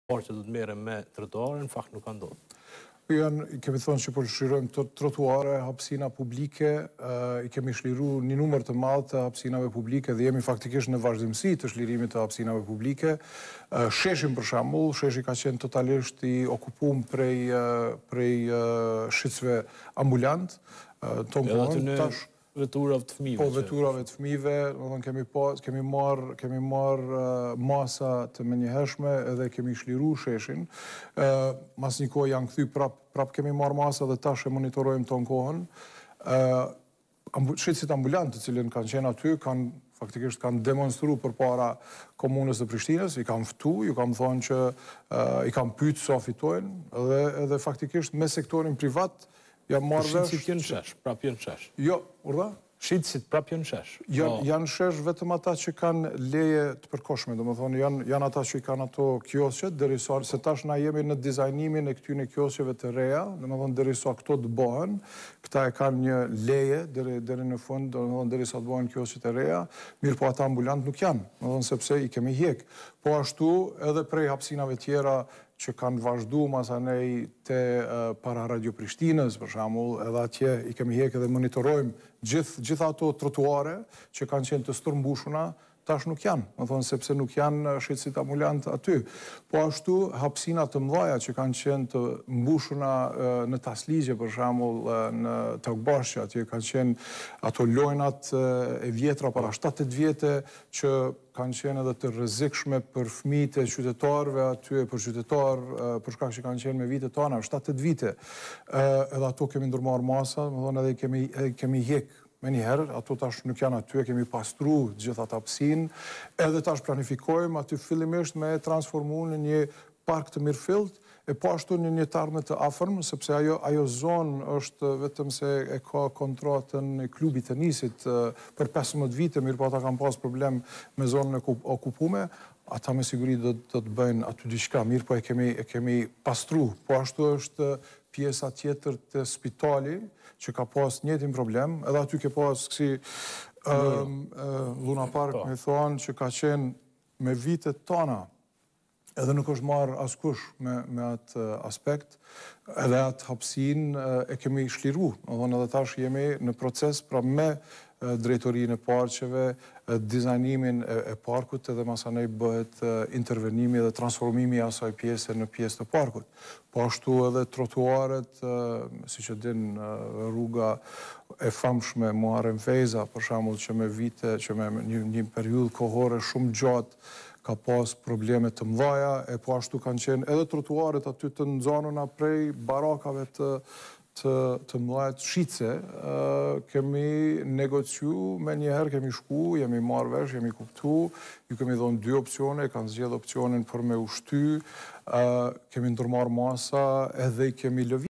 Rama: E kemi shliru sheshin nga shitësit ambulantë | T7
T7 Intervistë me kryetarin e Prishtinës, Përparim Rama/ Pressing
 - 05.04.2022